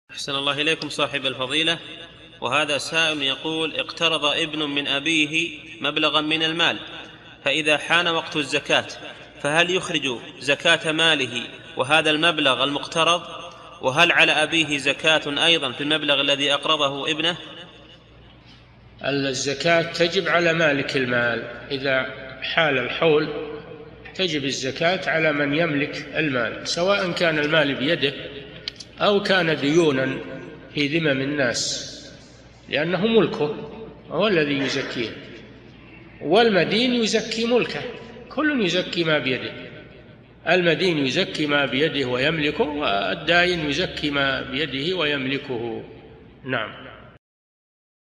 [21 -954] اقترض ابن من أبيه، فإذا حان وقت الزّكاة، هل يُخرج زكاة ماله، وهذا المبلغ المقترض..؟
-[21 /954] السؤال: اقترض ابنٌ من أبيه مبلغًا من المال، فإذا حان وقت الزَّكاة، فهل يُخرج زكاة ماله، وهذا المبلغ المُقترض، وهل على أبيه زكاةٌ أيضًا في المبلغ الذي أقرضه ابنه؟
الجواب: الزكاة تجب على مالك المال إذا حال الحول تجب الزكاة على مَن يملك المال، سواءً كان المال بيده، أو كان ديونًا في ذمم الناس، لأنه مُلكه وهو الذي يُزكيه، والمَدين يُزكي مُلكه، كلٌ يُزكي ما بيده، المدين يُزكي ما بيده ويملكه، والدائن يُزكي ما بيده ويملكه.
للاشتراك في قناة الشيخ صالح الفوزان
فتاى شرح منظومة الآداب ( 945 ) مقطع

للاستفسار والإقتراحات والدعم
رئيس الفريق
لمشروع كبار العلماء